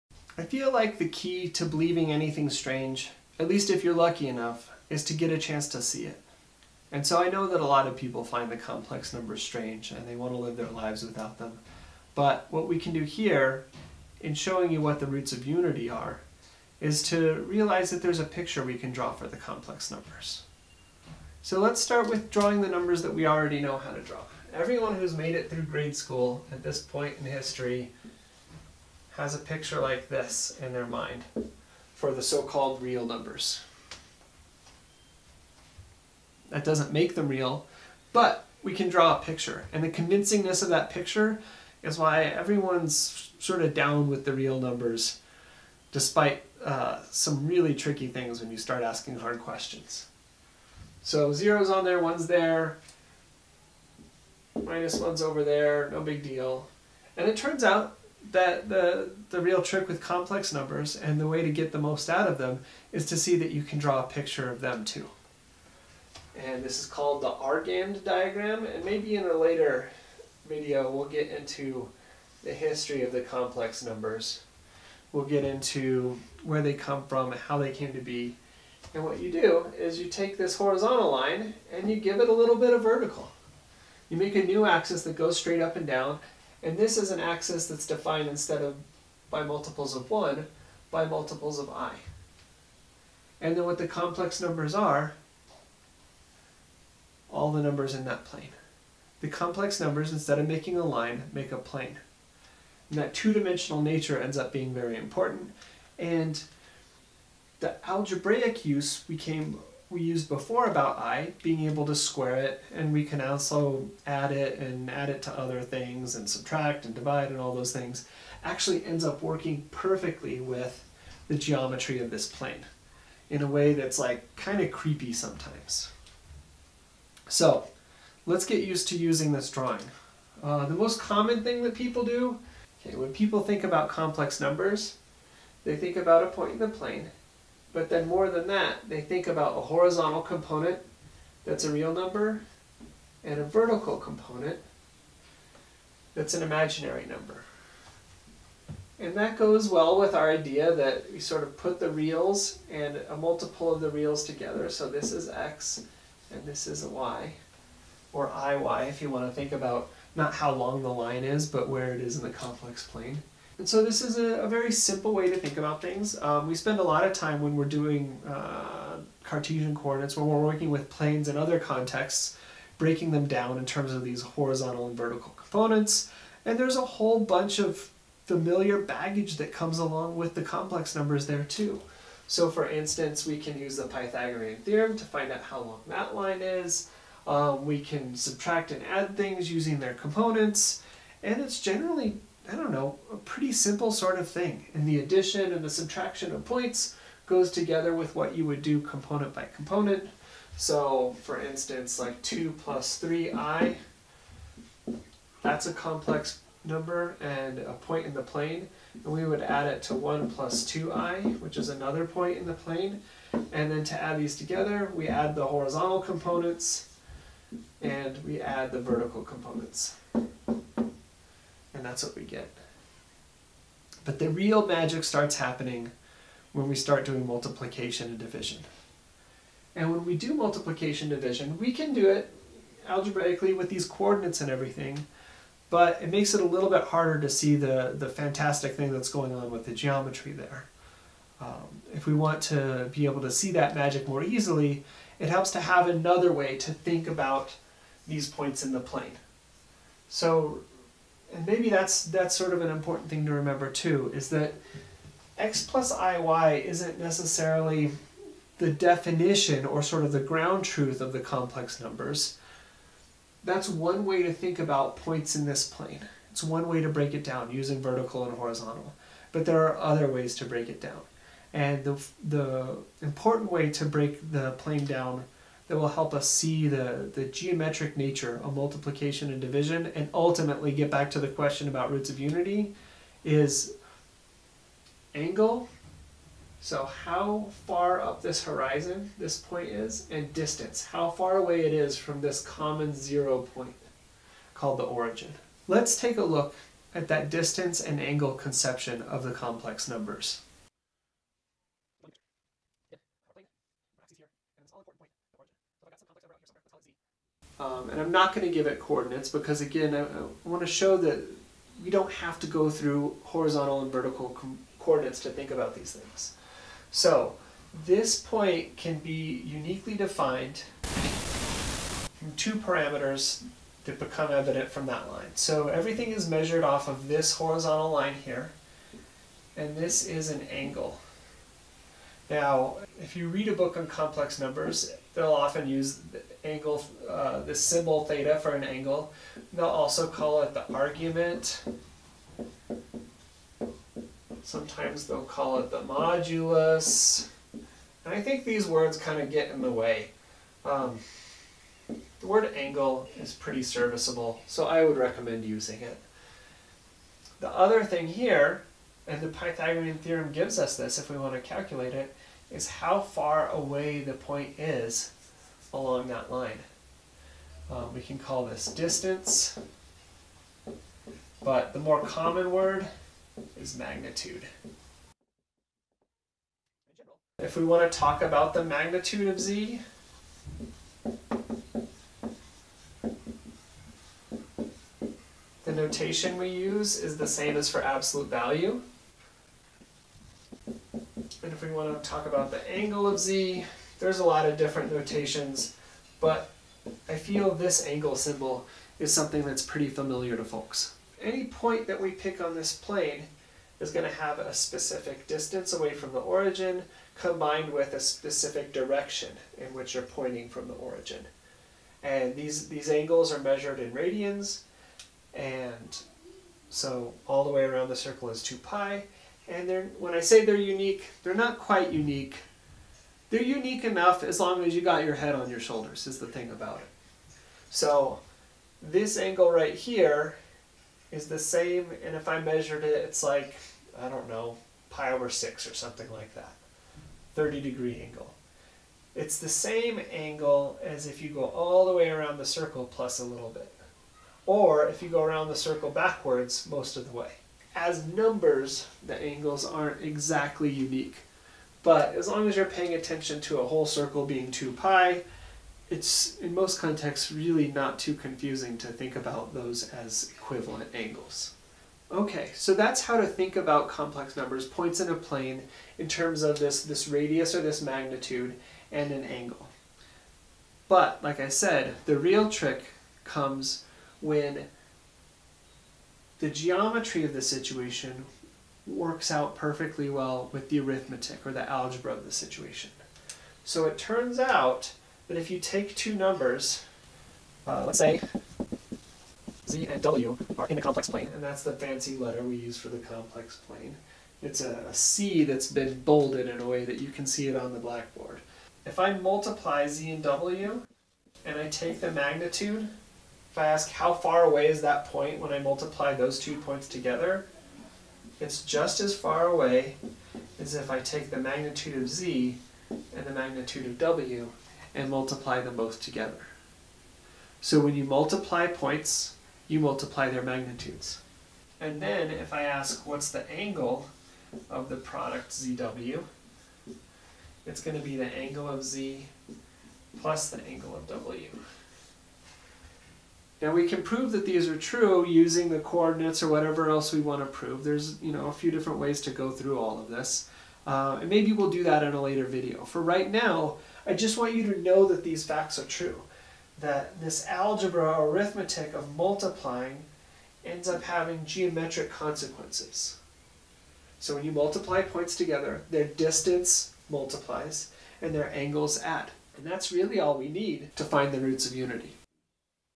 Roots of Unity 02 coordinate and angle magnitude arithmetic of complex numbers
The complex numbers are more reasonable seeming if we draw their picture: a plane of points. Once we do this, we can think about them in terms of x and y coordinates, a real and imaginary part. Then a lot of the intuition we have about adding and coordinate geometry comes into play. Even better though, if we think about points in the plane as having magnitude and angle, we can tie the arithmetic of multiplication to the geometry of magnitudes and angles. This is the key to finding the roots of unity.